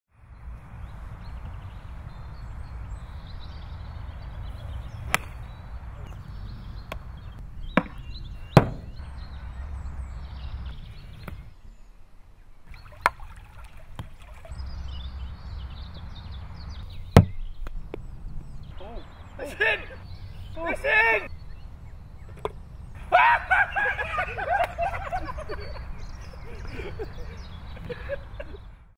TIM REAM | The Ryder Cup beckons?
So, it turns out Tim Ream is pretty good at golf. Here he is at his very best alongside team-mates Eidur Gudjohnsen and Ross Fitzsimons at the club's recent golf day.

An unbelievable hole in one!